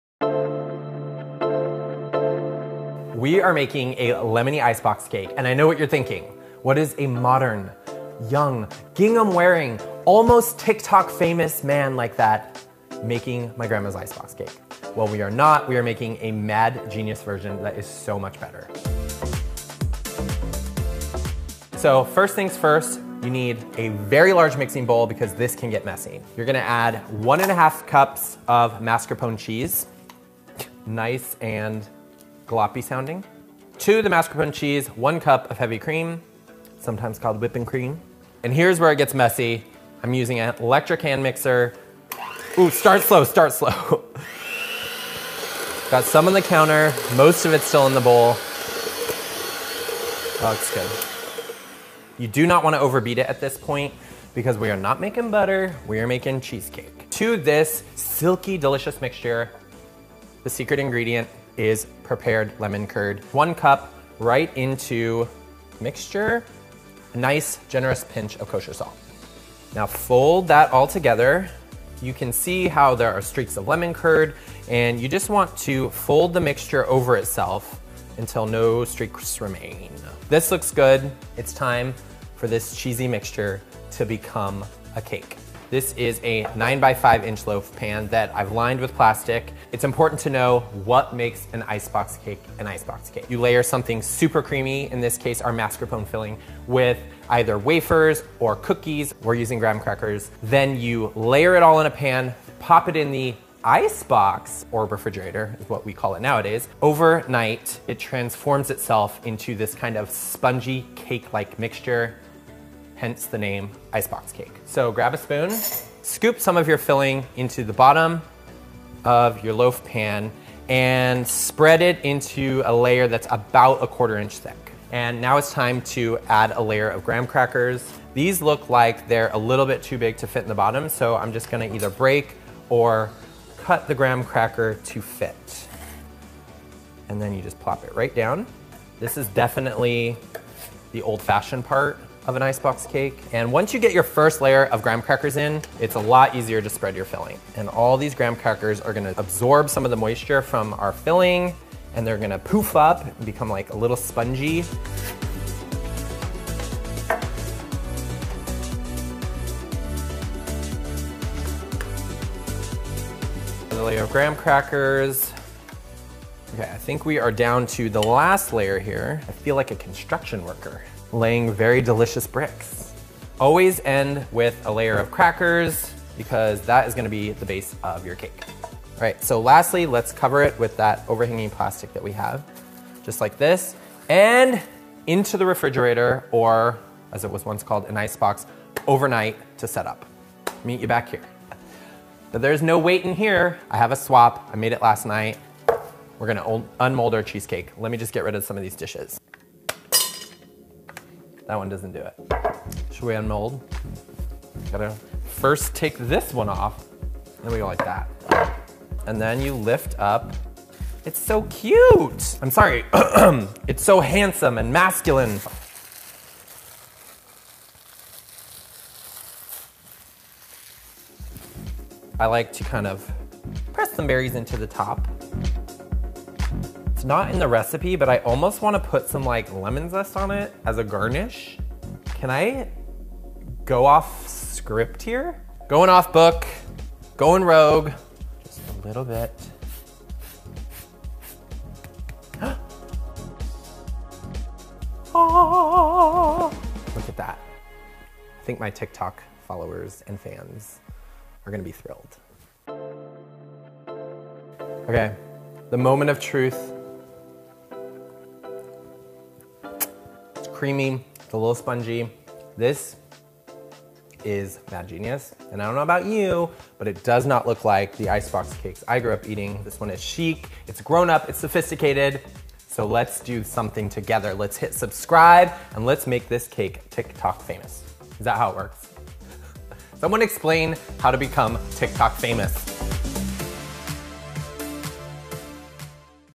Lemony Layered Cheesecake
The smart trick here is using prepared ingredients to compose this tangy icebox cake.